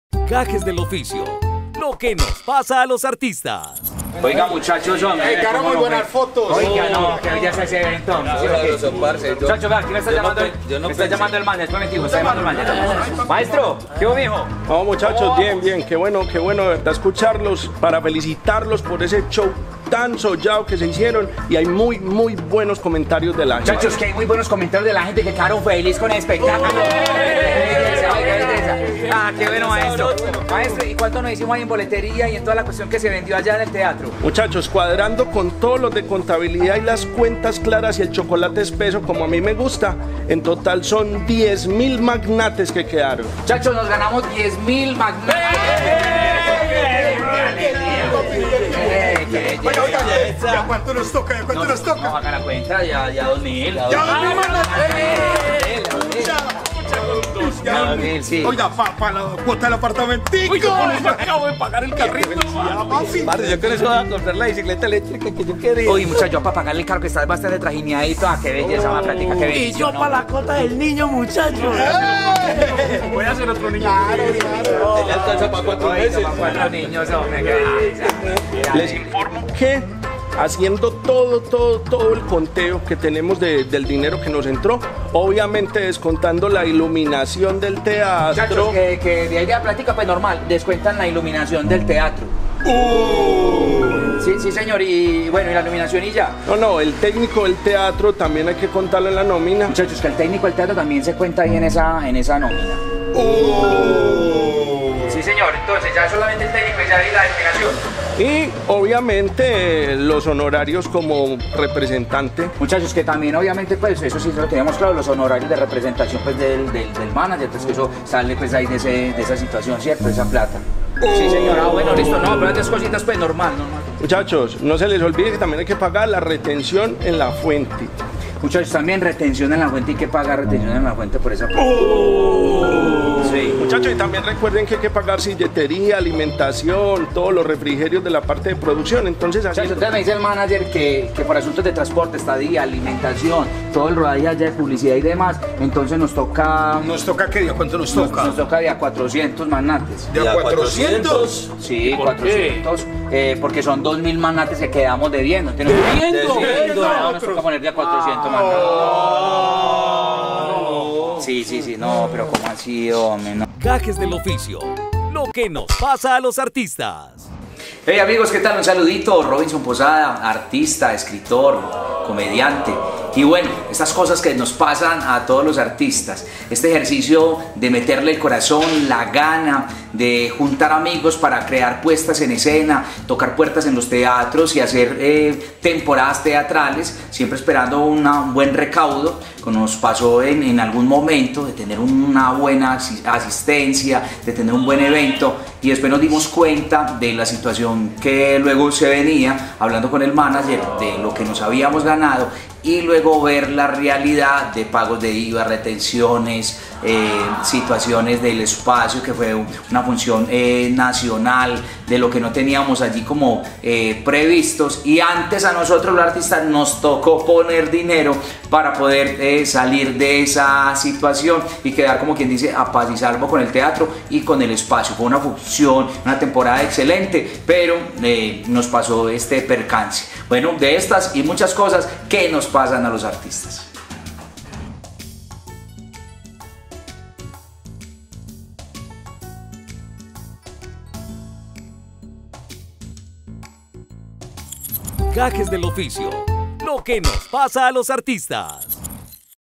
¡Nos ganamos un billete! - Gajes del oficio - EP 08
Las cosas de ser emprendedor y sobre todo, un artista emprendedor en Colombia, si no me creen, aquí les dejo, este y todos los viernes a las 12 del medio día un nuevo episodio de GajesDelOficio

y en procrédito.

Libretos y dirección: Robinson Posada